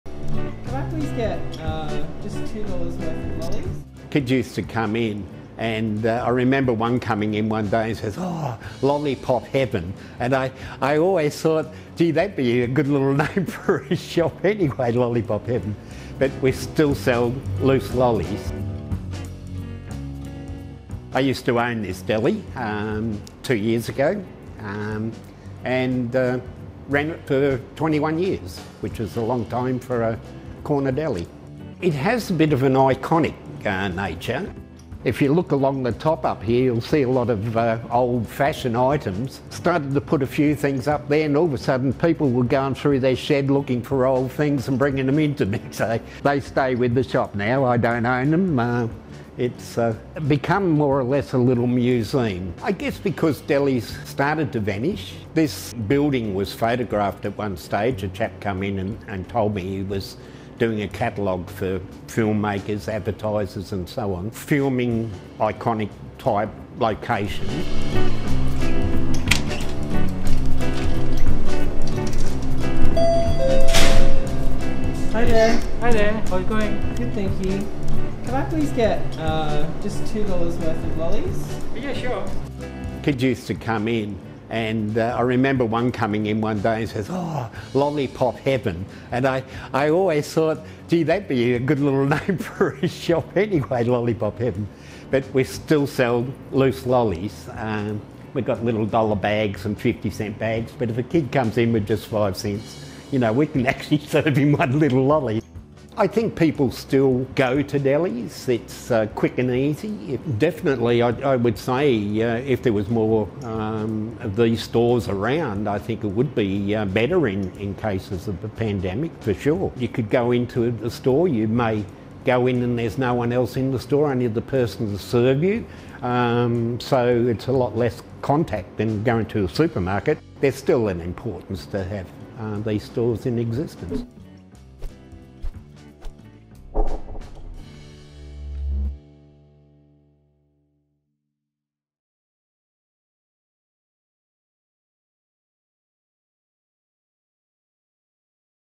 130-year-old corner store keeps traditional experience alive | Amazing Australia | ABC Australia
A neighbourhood corner store in the outback NSW town of Broken Hill is a business thriving through the ages. The building has had seven generations of owners and multiple business models, as well as being a creative backdrop for local and interstate film crews. Loyal and lifelong customers have used its vintage aesthetic — which features the old Shelley's Cordial sign — as a colourful and awe-inspiring hallmark for the community that surrounds it.